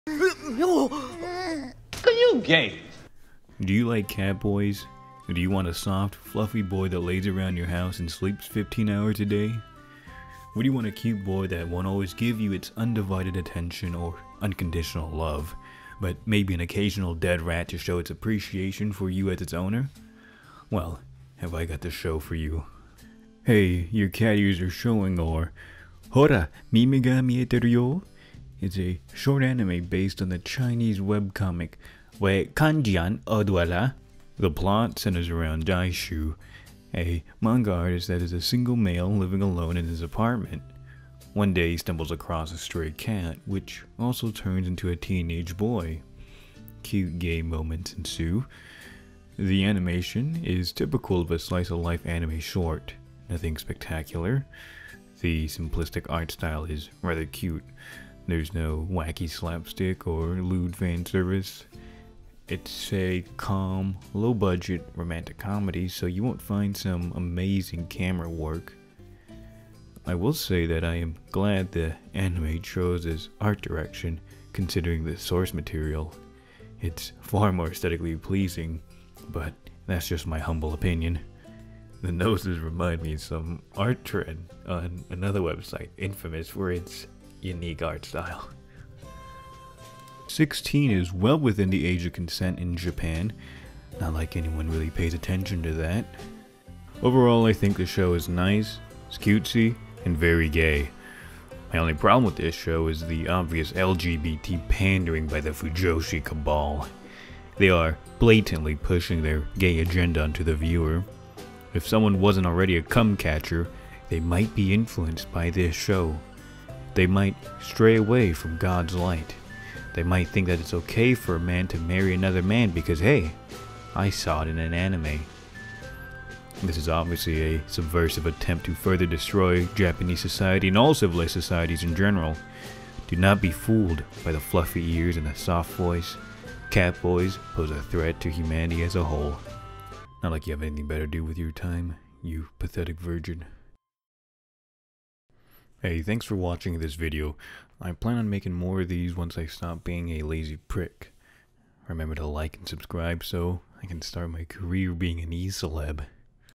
Anime Review: Hora Mimi (Hey Your Cat Ears Are Showing)
Hora, Mimi ga Mieteru yo! is the cute story of the daily life between a guy and his cat-boy roommate. However, it is a far deeper tale about how one man can delve into a sick, perverted relationship. The depravity of such an act would make even the most hardcore viewers grimace. In this video I will give my honest opinion about the inter-species romance.